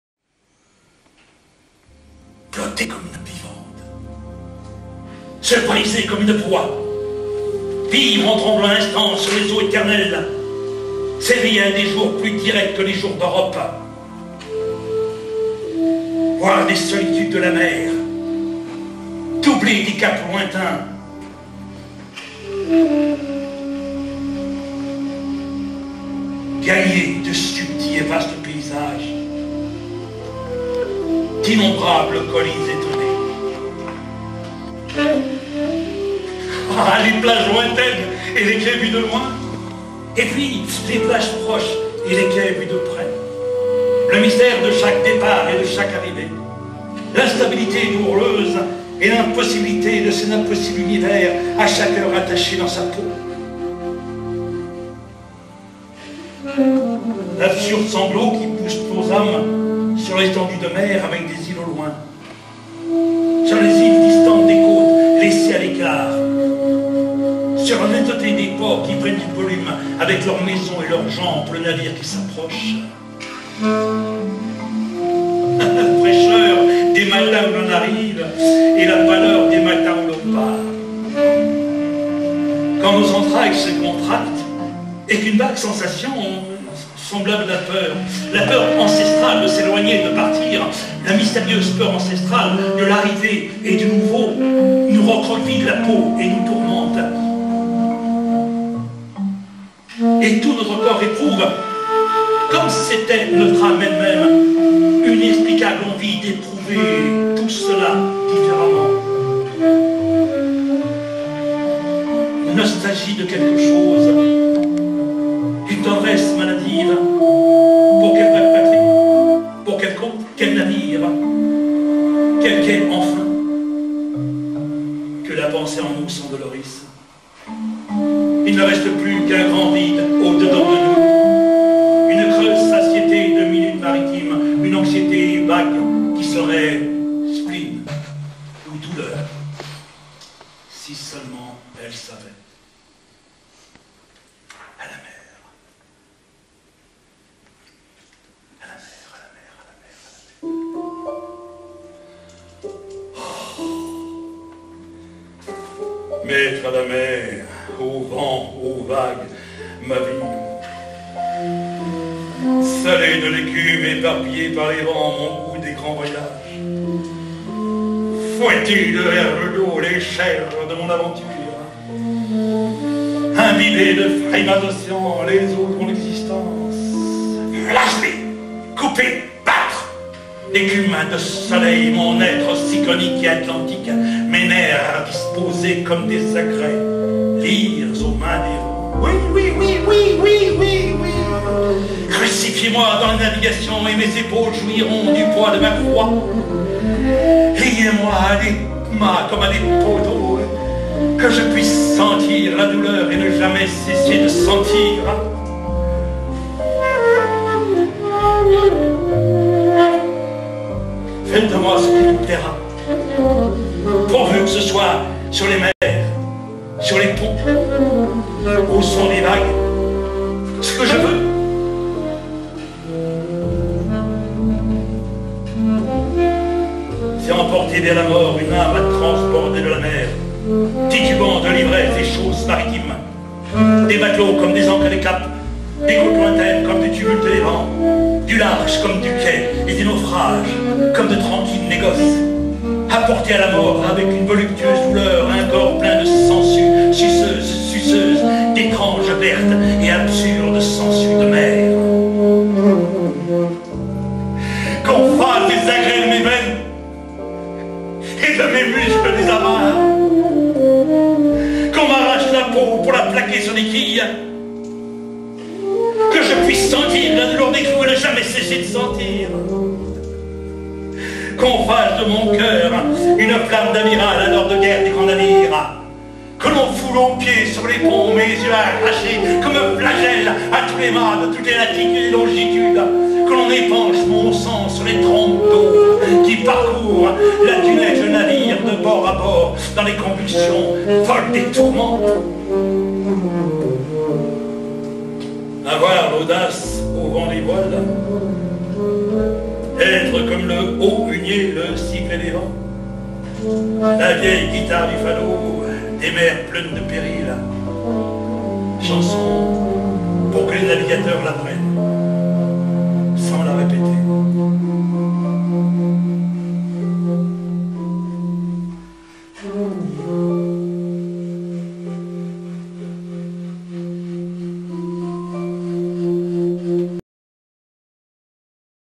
Sur le Quai (Ode Maritime) de Fernando Pessoa, avec Ivan Romeuf et Jean Jacques Lion.
Mise en scène, interprétation Ivan Romeuf. Musique : Jean Jacques Lion composition et saxophones. Assistante : Marie Line Rossetti. Création Lumière : Marie Lefèvre..
Dans le cadre de la carte blanche de Jean Jacques Lion, au théâtre de Lenche mai 2014.